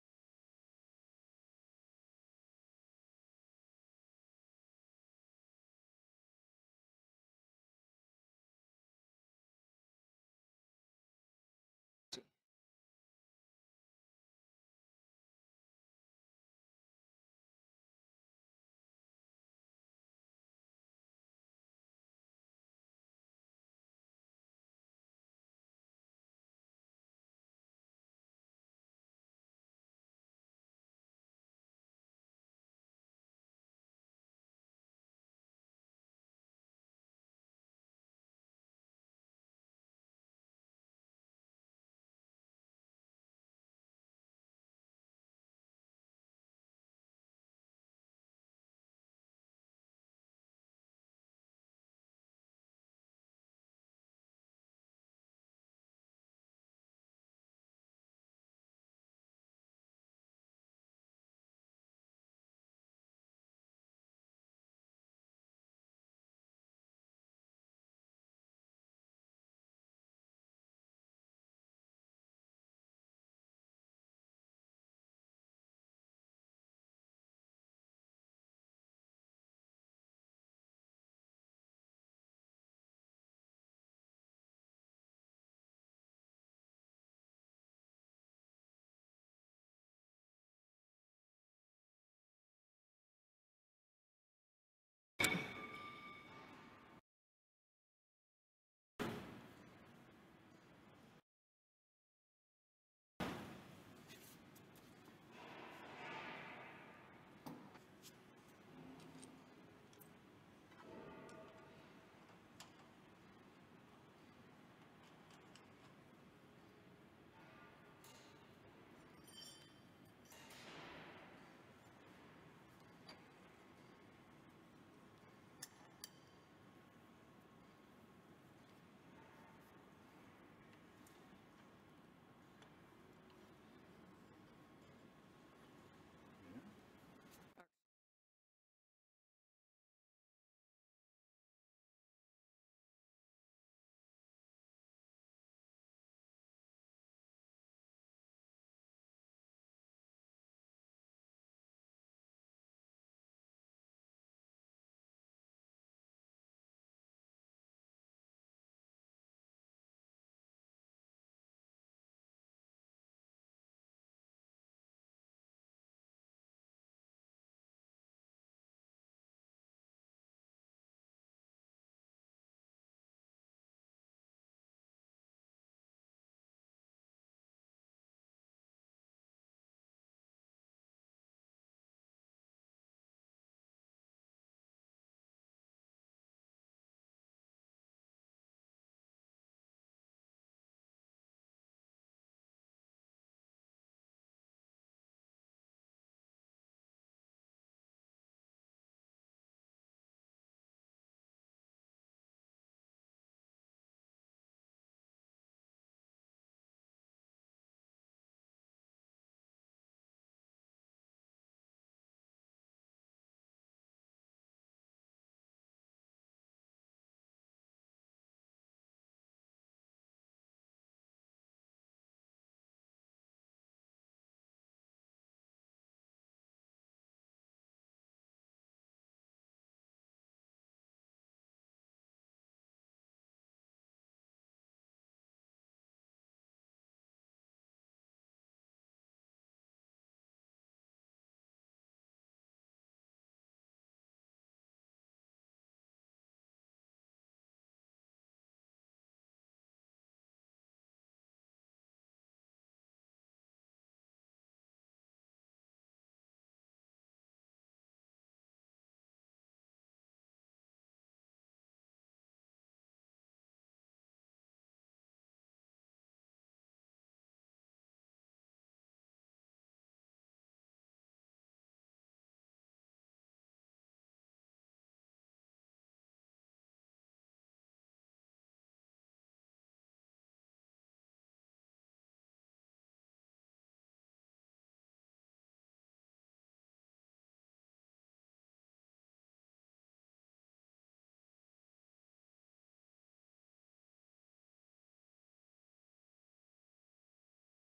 How to maintain the brake and bumper of a Betrac, Berg Hortimotive
In this video we want to show you how you can maintain the brake and bumper of a Betrac AGV. If you want to get some help you can contact one of our service partners, here you can find an overview of all our service partners;

Do you want to stay informed about our new videos?